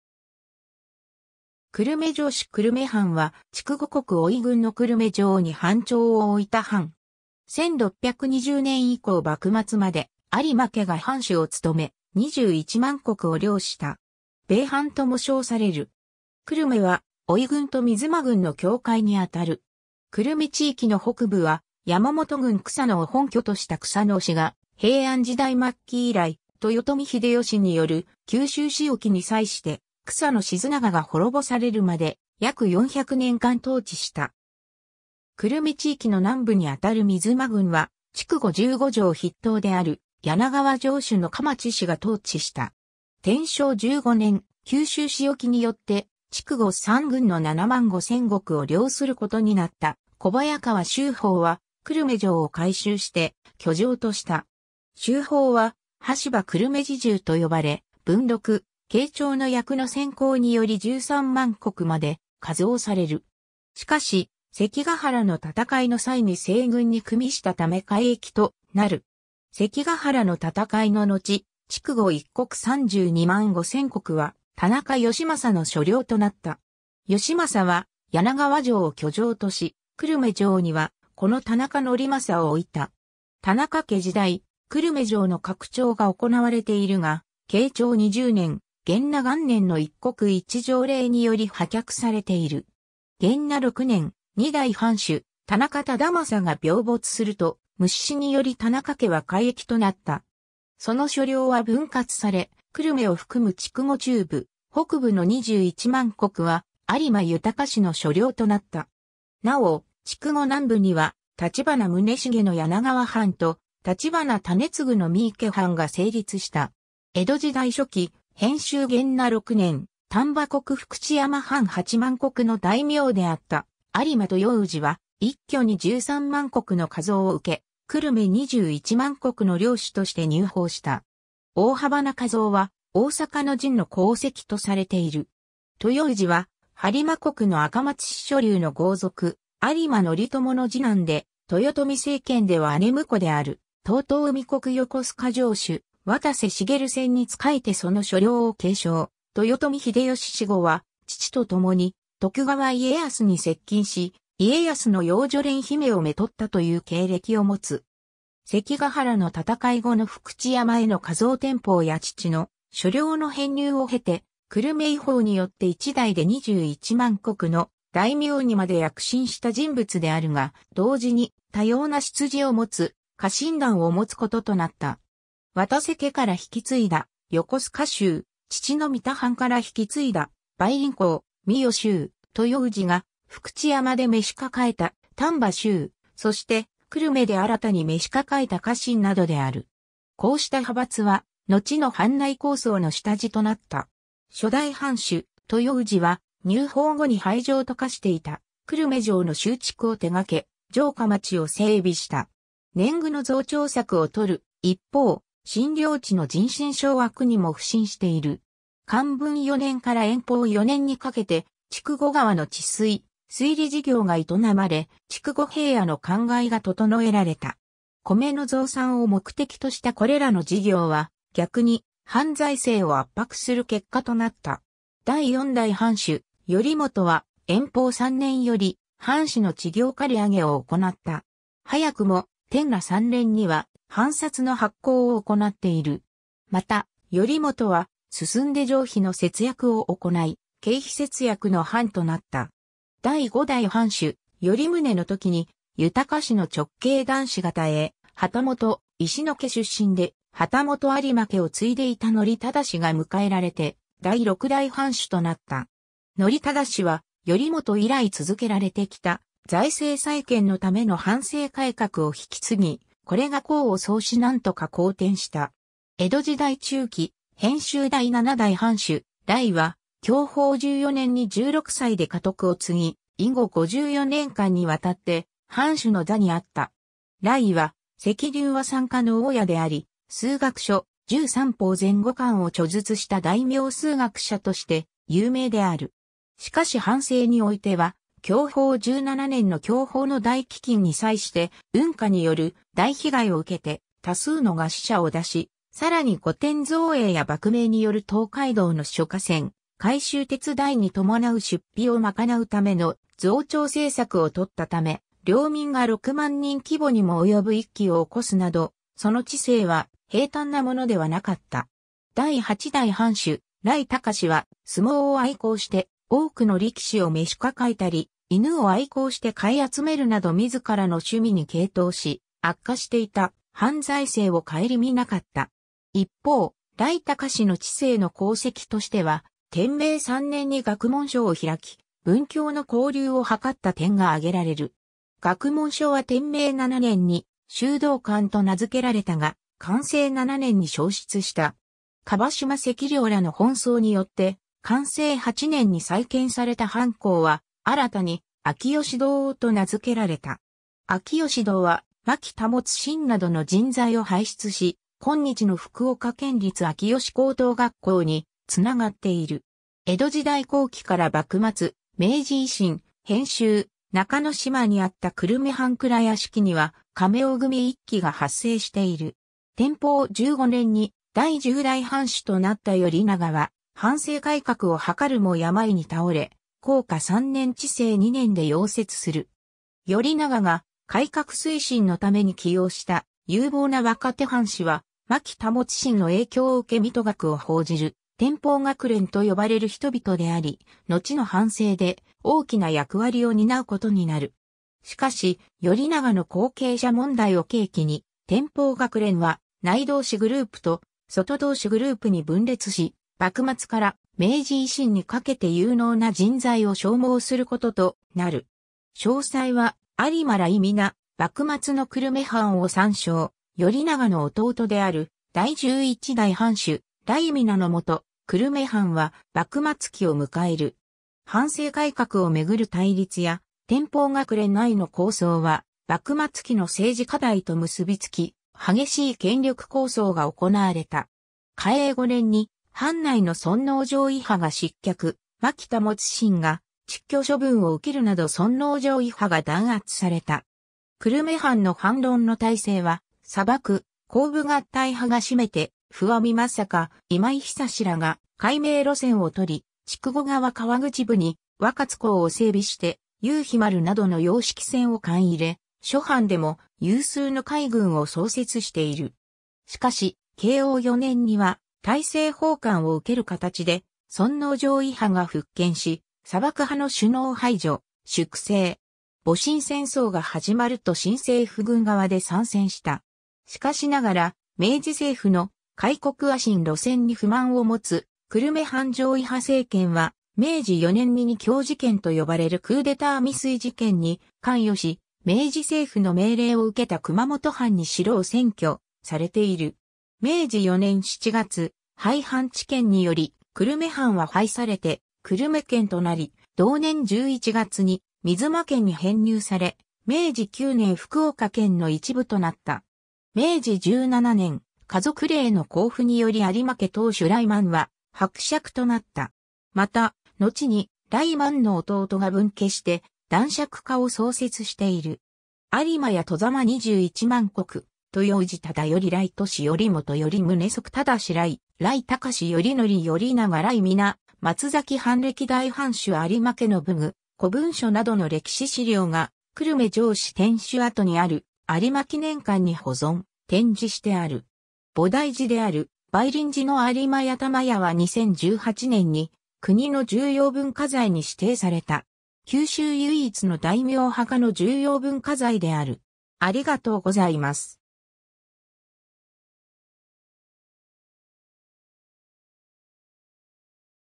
久留米藩
久留米城址 久留米藩（くるめはん）は、筑後国御井郡の久留米城（現在の福岡県久留米市）に藩庁を置いた藩。
1620年以降幕末まで有馬家が藩主を務め、21万石を領した。
 米藩（べいはん）とも称される。
 久留米は、御井郡と三潴郡の境界にあたる。
久留米地域の北部は、山本郡草野（現在の久留米市草野町）を本拠とした草野氏が、平安時代末期以来、豊臣秀吉による九州仕置に際して草野鎮永が滅ぼされるまで約400年間統治した。
久留米地域の南部にあたる三潴郡は筑後十五城筆頭である柳川城主の蒲池氏が統治した。
 天正15年（1587年）、九州仕置によって筑後3郡の7万5千石を領することになった小早川秀包は、久留米城を改修して居城とした。
秀包は「羽柴久留米侍従」と呼ばれ、文禄・慶長の役の戦功により13万石まで加増される。
しかし、関ヶ原の戦いの際に西軍に与したため改易となる。
 関ヶ原の戦いののち、筑後一国32万5000石は田中吉政の所領となった（柳河藩参照）。
吉政は柳河城を居城とし、久留米城には子の田中則政（吉信）を置いた。
田中家時代、久留米城の拡張が行われているが、慶長20年/元和元年（1615年）の一国一城令により破却されている。
 元和6年（1620年）、2代藩主田中忠政が病没すると、無嗣子により田中家は改易となった。
その所領は分割され、久留米を含む筑後中部・北部の21万石は有馬豊氏の所領となった。
なお、筑後南部には立花宗茂の柳河藩（10万9000石）と、立花種次（宗茂の甥）の三池藩（1万石）が成立した。
 江戸時代初期[編集] 元和6年（1620年）、丹波国福知山藩8万石の大名であった有馬豊氏は、一挙に13万石の加増を受け、久留米21万石の領主として入封した。
大幅な加増は大坂の陣の功績とされている（なお、有馬家末裔の有馬頼底は「大した働きもしていないのに13万石加増になったのは不可思議である」旨の発言をしている）。
 豊氏は、播磨国の赤松氏庶流の豪族・有馬則頼の二男で、豊臣政権では姉婿である遠江国横須賀城主渡瀬繁詮に仕えてその所領を継承、豊臣秀吉死後は父とともに徳川家康に接近し、家康の養女蓮姫を娶ったという経歴を持つ。
関ヶ原の戦い後の福知山への加増転封や父の所領（三田藩）の編入を経て、久留米移封によって一代で21万石の大名にまで躍進した人物であるが、同時に多様な出自を持つ家臣団を持つこととなった。
渡瀬家から引き継いだ「横須賀衆」、父の三田藩から引き継いだ「梅林公御代衆」、豊氏が福知山で召し抱えた「丹波衆」、そして久留米で新たに召し抱えた家臣などである。
こうした派閥は、のちの藩内抗争の下地となった。
 初代藩主・豊氏は、入封後に廃城と化していた久留米城の修築を手がけ、城下町を整備した。
年貢の増徴策を取る一方、新領地の人心掌握にも腐心している。
 寛文4年（1664年）から延宝4年（1676年）にかけて、筑後川の治水・水利事業が営まれ、筑後平野の灌漑が整えられた。
米の増産を目的としたこれらの事業は、逆に藩財政を圧迫する結果となった。
第4代藩主・頼元は延宝3年（1675年）より藩士の知行借り上げを行った。
早くも天和3年（1681年）には藩札の発行を行っている。
また、頼元はすすんで冗費の節約を行い、経費節約の範となった。
 第5代藩主頼旨の時に豊氏の直系男子が絶え、旗本石野家（赤松氏一門）出身で旗本有馬家（則頼の四男にはじまる家）を継いでいた則維が迎えられて第6代藩主となった。
則維は、頼元以来続けられてきた財政再建のための藩政改革を引き継ぎ、これが功を奏し何とか好転した。
 江戸時代中期[編集] 第7代藩主・頼徸は享保14年（1729年）に16歳で家督を継ぎ、以後54年間にわたって藩主の座にあった。
頼徸は関流和算家の大家であり、数学書『拾璣算法（しゅうきさんぽう）』全5巻を著述した大名数学者として有名である。
しかし藩政においては、享保17年（1732年）の享保の大飢饉に際してウンカによる大被害を受けて多数の餓死者を出し、さらに御殿造営や幕命による東海道の諸河川改修手伝いにともなう出費を賄うための増徴政策をとったため、領民が6万人規模にも及ぶ一揆を起こすなど、その治世は平坦なものではなかった。
 第8代藩主・頼貴は、相撲を愛好して多くの力士を召し抱えたり（雷電爲右エ門との勝負で知られる横綱小野川喜三郎や筑後出身の鯱和三郎は久留米藩の抱え力士である）、犬を愛好して買い集めるなど自ら...